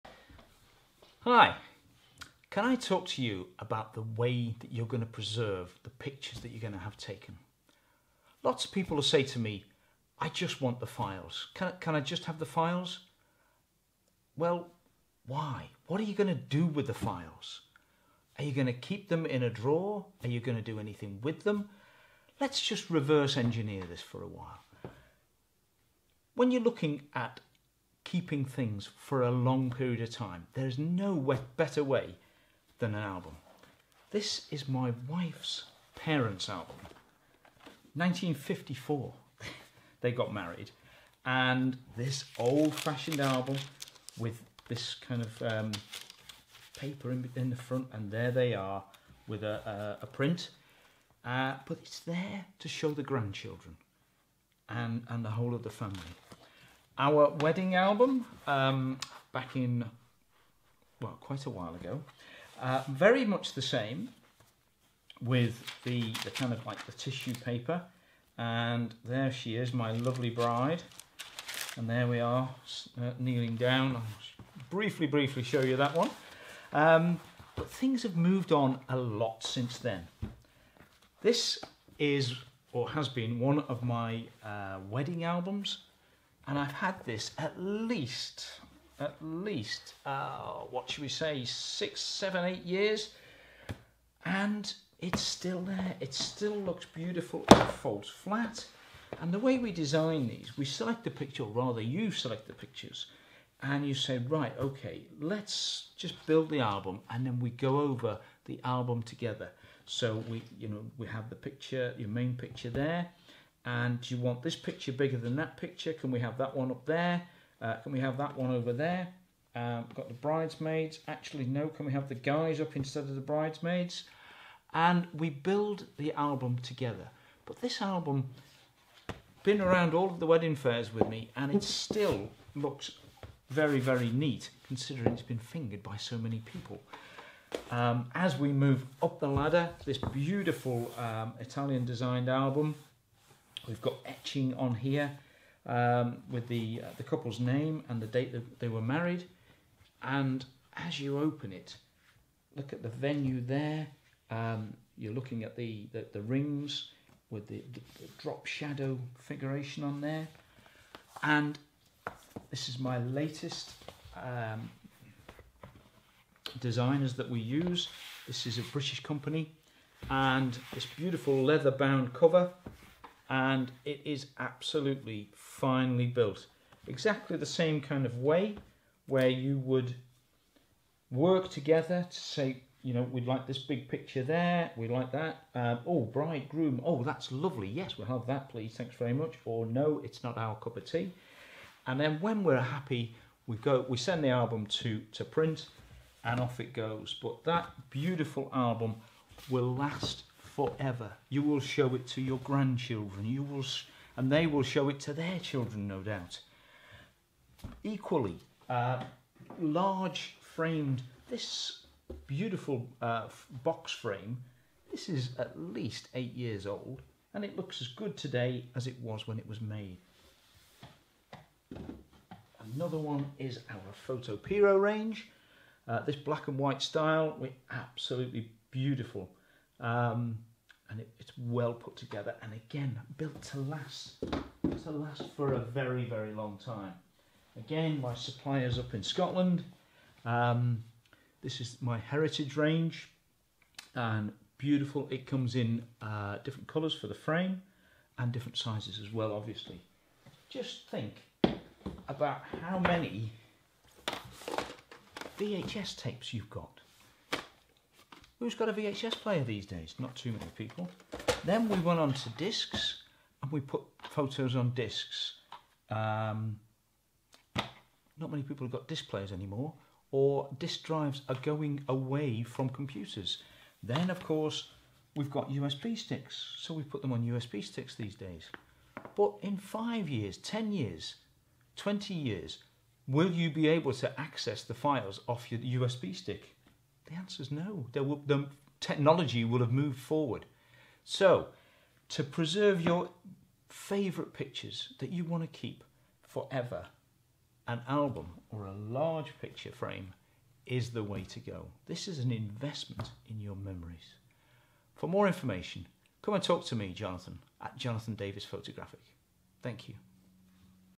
How to preserve your memories
As a portrait photographer I often get asked "what's next" ??
Can I have the files? what are you going to do with the files?
I have the answers for you, with some very special wall art products and bespoke albums to help that you can treasure your memories for many years to come and share with your family for generations along the way.
A range of wall art products to suit different tastes and budgets from small 7" x 5" framed prints to giant 60" metal art